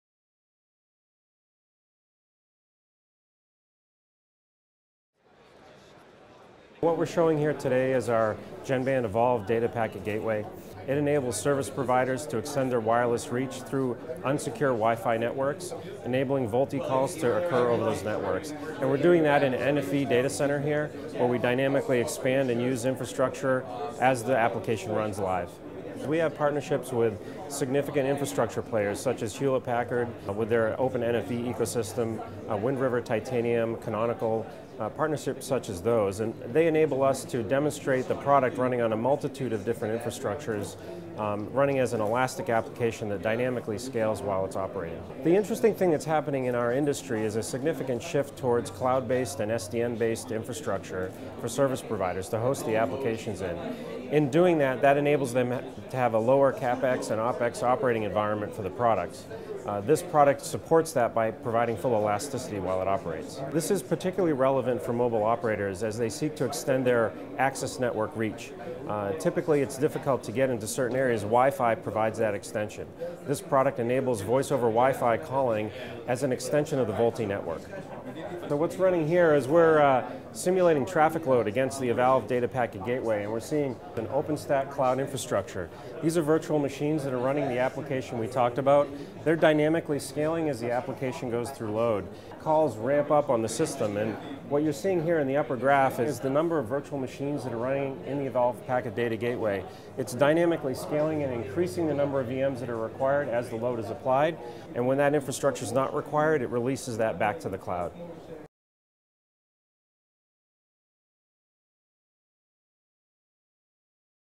VoLTE over WiFi: economically extending reach
Originally Published on TelecomTV.com 17 Mar 2016

GENBAND’s Evolved Data Packet Gateway lets voice service providers extend Voice over LTE (VoLTE) calls across unsecured WiFi networks - especially into environments (like basements) that the mobile network can have difficulty reaching. GENBAND has multiple NFV partnerships and above demonstrates the elasticity of its VoLTE application, which dynamically scales up with load by increasing the number of ‘virtual machines’ and then releasing the processing power back to the cloud when it’s no longer required.
Filmed at: Mobile World Congress, Barcelona, 2016